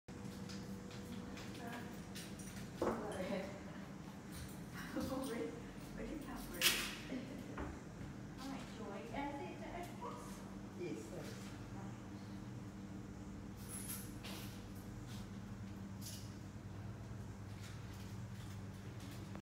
Silks and Jewellery
Hand dyed eco silk scarfs, handmade jewellery, word art blocks, traditional weaving, and so much more - here at Estuary arts Centre